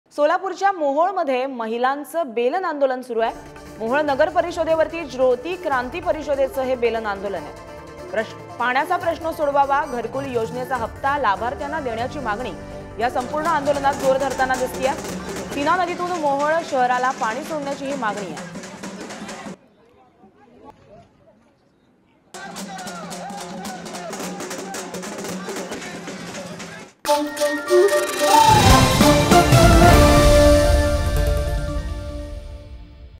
Pudhari News | पाणी प्रश्नासह इतर मागण्यांसाठी सोलापुरच्या मोहोळमध्ये महिलांचं अनोखं बेलन आंदोलन
Pudhari News | पाणी प्रश्नासह इतर मागण्यांसाठी सोलापुरच्या मोहोळमध्ये महिलांचं अनोखं बेलन आंदोलन | Solapur News